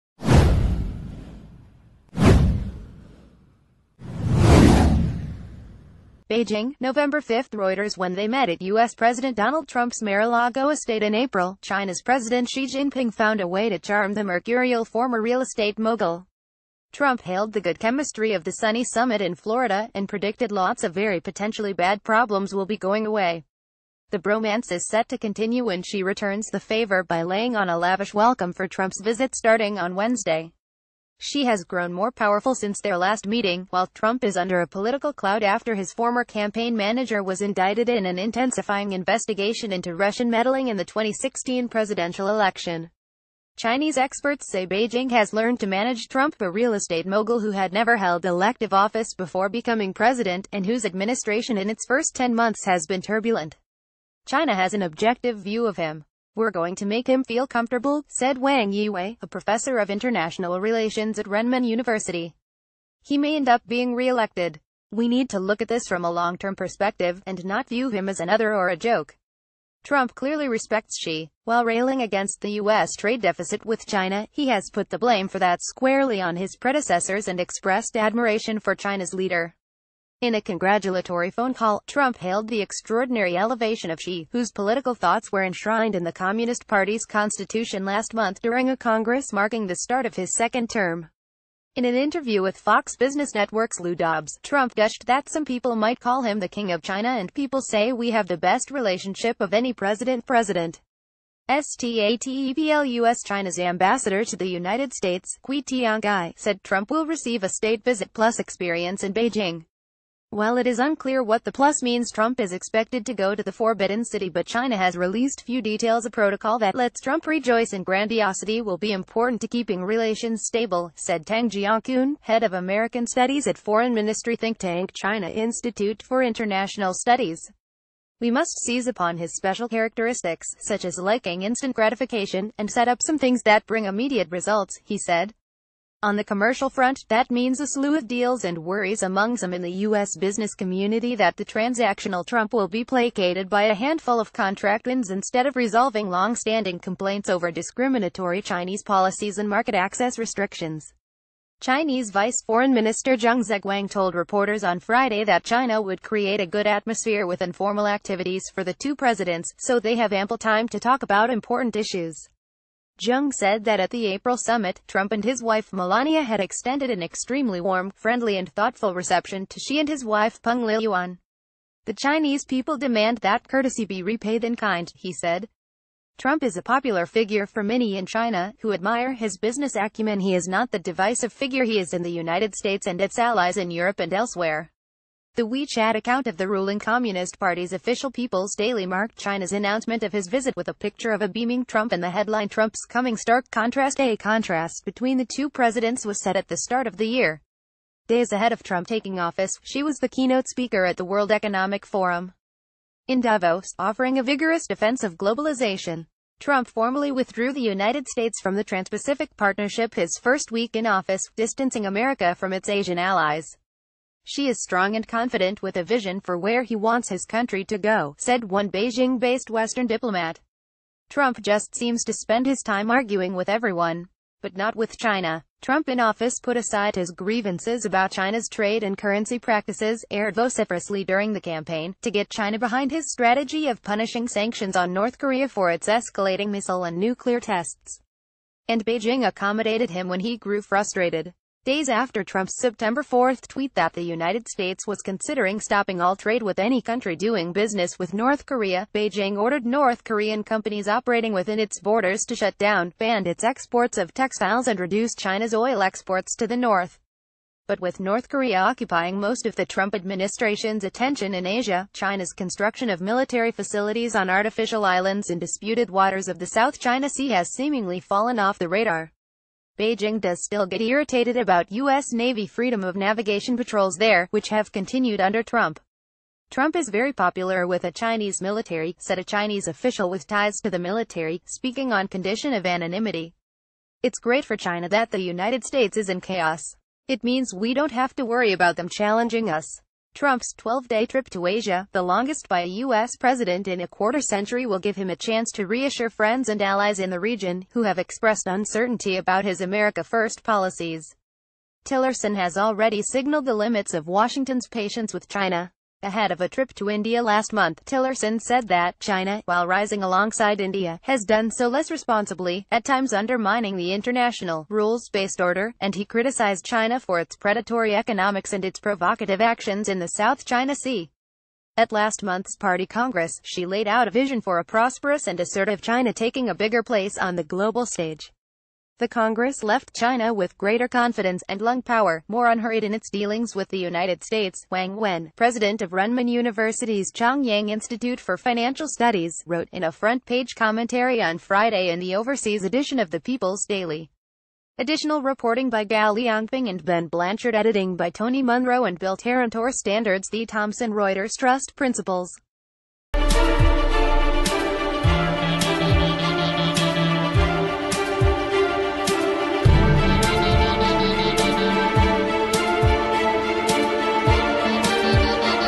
Breaking News | Riding high, xi looks to soothe trump as u.s. pressures china
Riding high, xi looks to soothe trump as u.s. pressures china

BEIJING, Nov 5 Reuters When they met at U.S. President Donald Trump’s MaraLago estate in April, Chin...